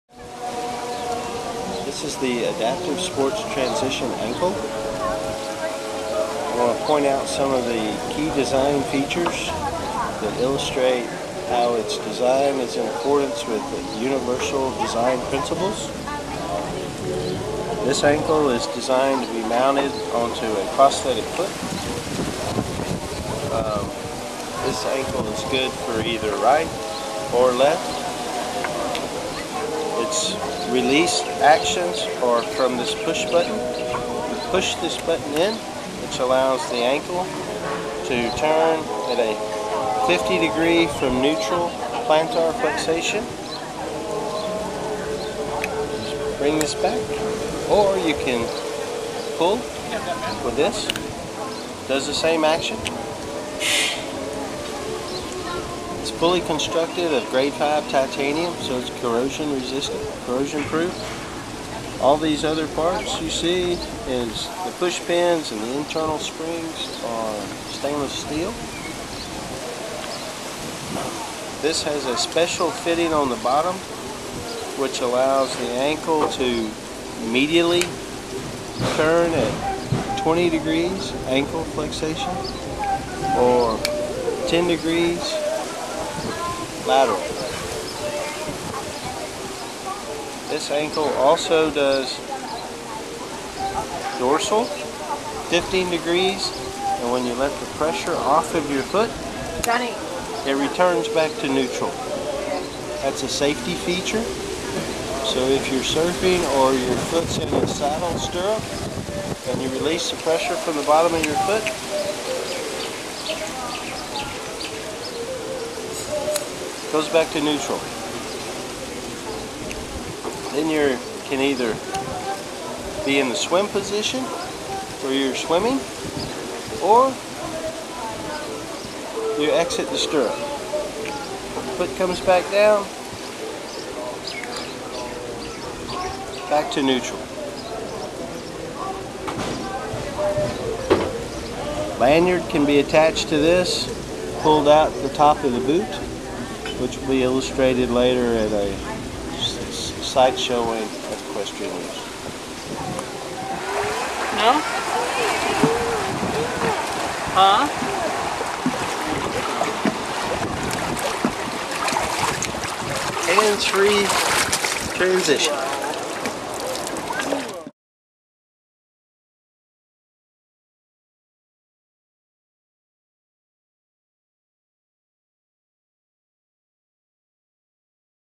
Adaptive Sports Transition Ankle.mov
The Adaptive Sports Transition Ankle is an ankle joint prosthetic component comprised of a central core element capable of attachment to an artificial leg, coupled with two side elements capable for attachment to an artificial foot.  The medial and lateral side elements envelop the central core lower region along a common central axis, and are coupled together by means of mechanical fastening and can be rotated relative to the central core and positively constrained to align in various orientations dependent of a specific grooved profile within each side element pair.  The ankle joint orientation is affected by engaging an anterior/posterior located mechanical linkage enacting an upward linear movement on a guide pin plunger, thus allowing the side elements to rotate by means of an imposed torque load reaching an intended orientation positively constrained within a grooved profile.  The load bearing elements shall be precision machined from Grade 5 Titanium material.  Mechanical linkages, fasteners, and springs shall be made from Series 300 Stainless Steel material.  All materials shall be surface treated by a chemical passivation process enabling maximum corrosion resistance for use in salt water and chemically treated swimming pools.